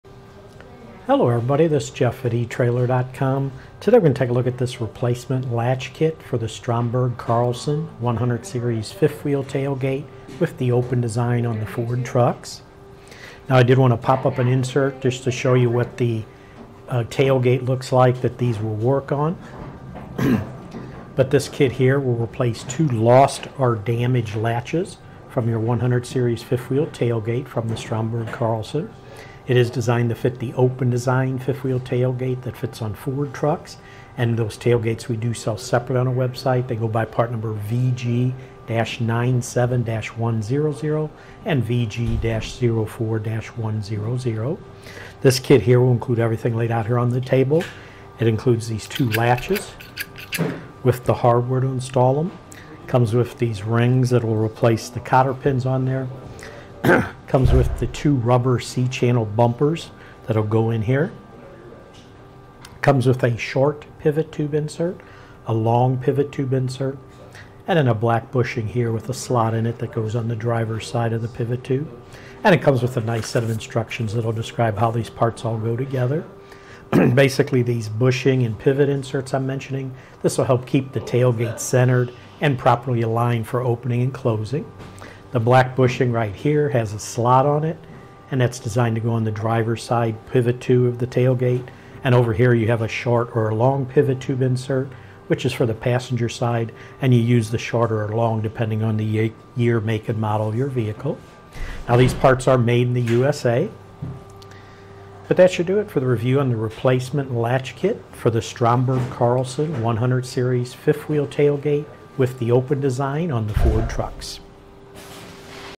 etrailer | Review of Stromberg Carlson Tailgate - Replacement Latch Kit - VG-97-100-1075
Click for more info and reviews of this Stromberg Carlson 5th Wheel Tailgate:

Hello everybody, this is Jeff at etrailer.com.  Today, we're gonna take a look at this replacement latch kit  for the Stromberg Carlson 100 Series fifth wheel tailgate  with the open design on the Ford trucks.  Now, I did wanna pop up an insert just to show you  what the tailgate looks like that these will work on,  but this kit here will replace two lost  or damaged latches from your 100 Series fifth wheel tailgate  from the Stromberg Carlson.  It is designed to fit the open design fifth wheel tailgate  that fits on Ford trucks,  and those tailgates we do sell separate on a website.  They go by part number VG-97-100  and VG-04-100.  This kit here will include  everything laid out here on the table.

It includes these two latches  with the hardware to install them.  Comes with these rings  that will replace the cotter pins on there.  Comes with the two rubber C channel bumpers  that'll go in here.  Comes with a short pivot tube insert,  a long pivot tube insert,  and then a black bushing here with a slot in it  that goes on the driver's side of the pivot tube.  And it comes with a nice set of instructions  that'll describe how these parts all go together.

Basically, these bushing  and pivot inserts I'm mentioning,  this will help keep the tailgate centered  and properly aligned for opening and closing.  The black bushing right here has a slot on it,  and that's designed to go on the driver's side,  pivot to the tailgate,  and over here you have a short or a long pivot tube insert,  which is for the passenger side,  and you use the short or long  depending on the year, make, and model of your vehicle.  Now, these parts are made in the USA.  But that should do it for the review on the replacement  latch kit for the Stromberg Carlson 100 Series  fifth wheel tailgate  with the open design on the Ford trucks.